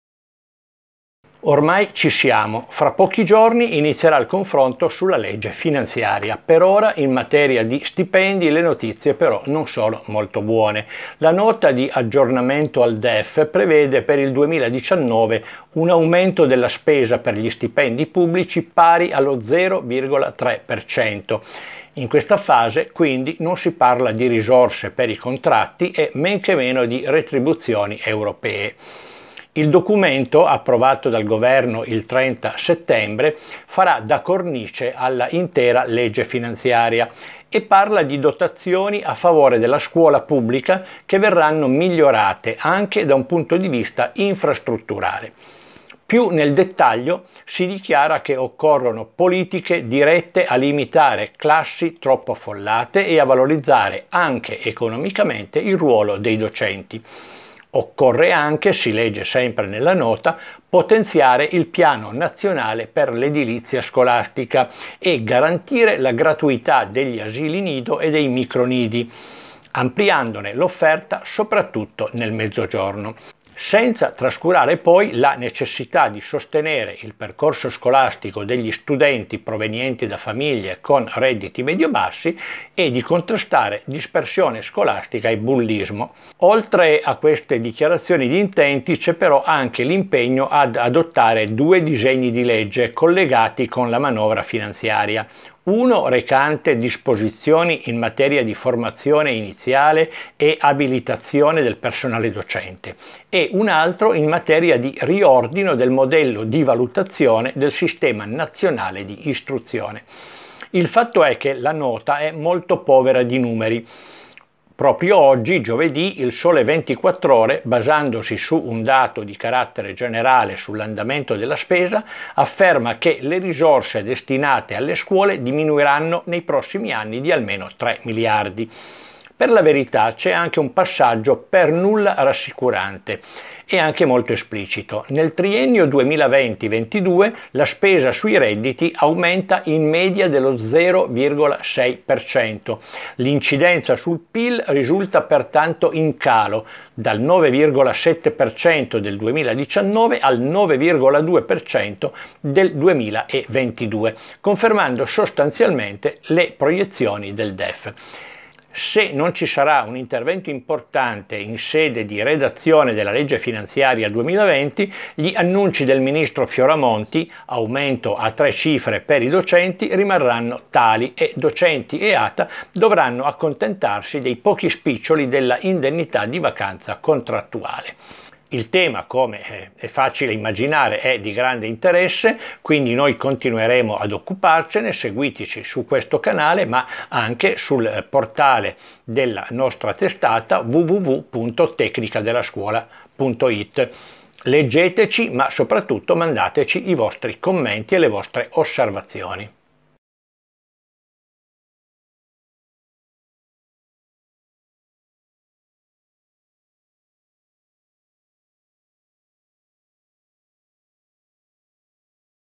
Legge di bilancio. Per ora niente soldi per gli stipendi
Il punto di Reginaldo Palermo
– Nota di aggiornamento DEF per gli stipendi pubblici. Si prevede un +0,3%
– Le intenzioni per la scuola sono: riduzione alunni per classe, valorizzazione docenti, lotta alla dispersione e al bullismo
– In arrivo due disegni di legge su reclutamento e valutazione del  sistema scolastico
– Stipendi pubblici in calo rispetto al PIL. Dal 9,7% del 2019 al 9,2% del 2022
– Per ora gli stipendi europei restano un annuncio